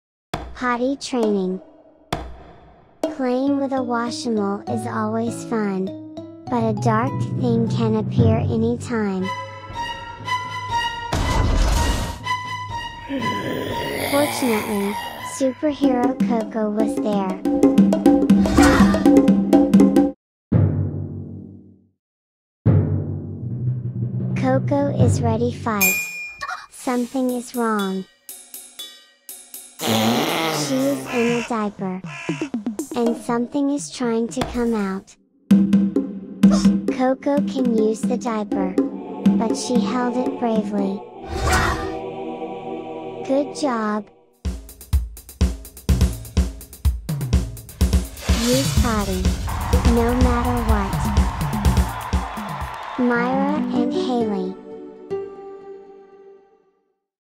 Potty Training | Use Potty, No Matter What | Myra and Haley
Potty training shorts clip of Myra and Haley
Providing a message of using a potty, no matter what.